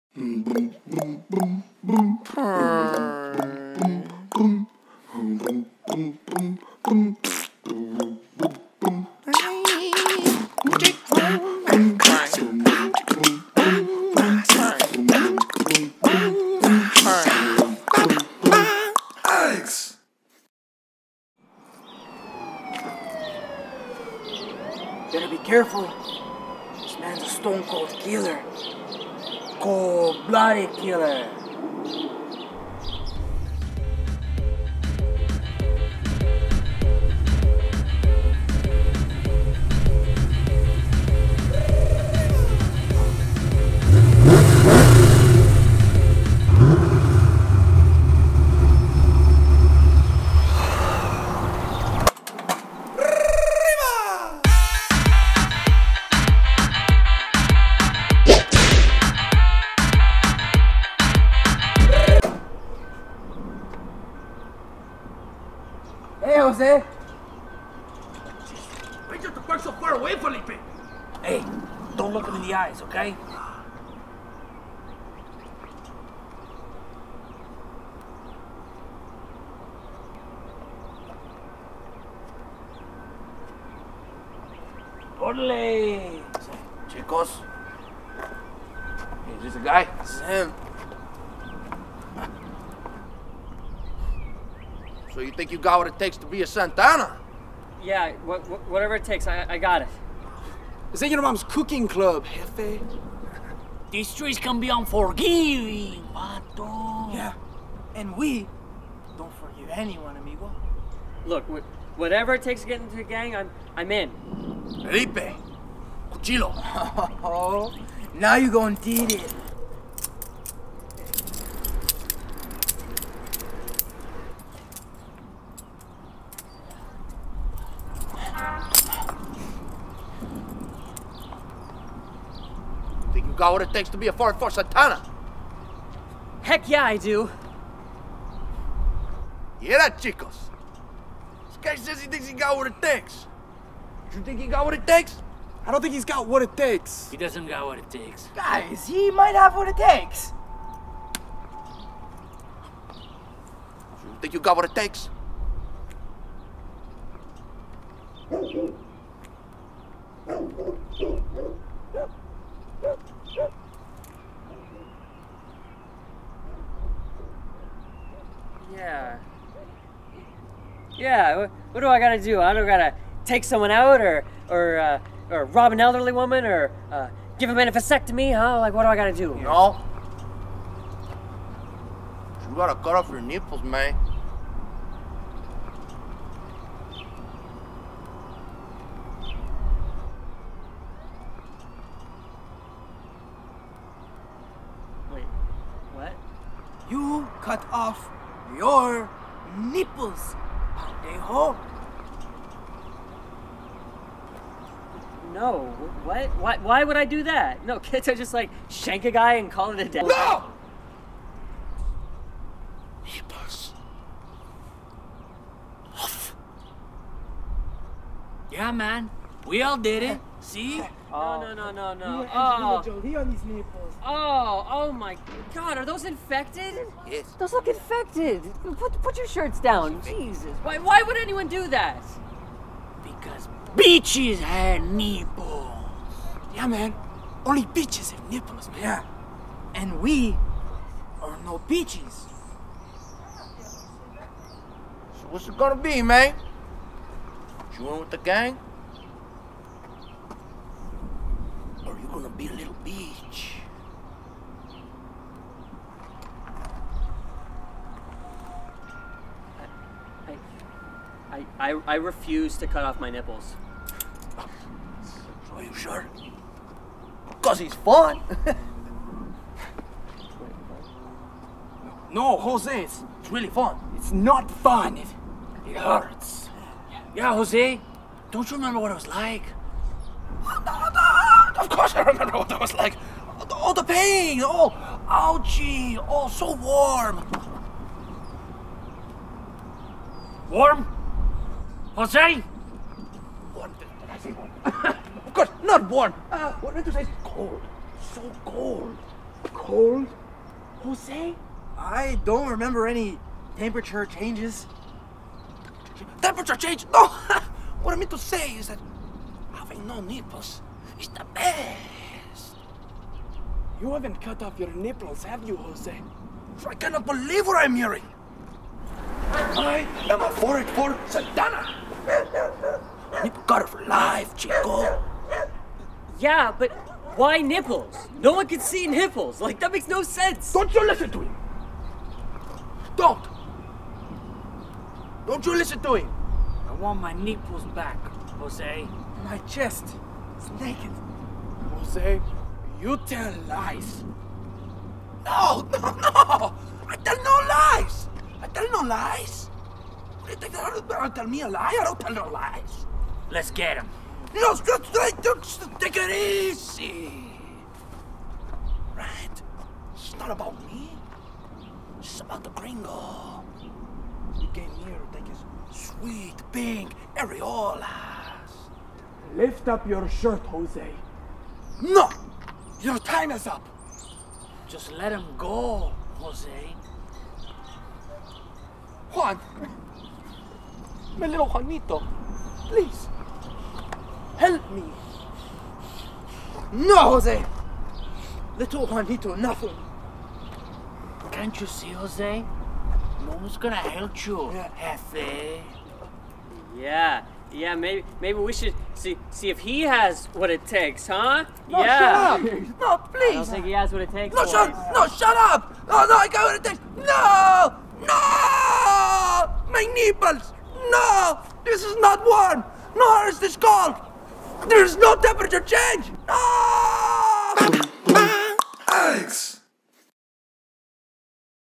The Initiation
If you want in the gang, you're gonna have to go through the initiation first.

"The Initiation" from Eggs Comedy

Starring:

Austen Alexander as Jose
Felipe
Nicholas Giorgi as Jesus
John-Paul Scarpelli as Jose
Tyler Mortimore as The Recruit

Produced by: Austen Alexander and David Koval

Written by: Austen Alexander, David Koval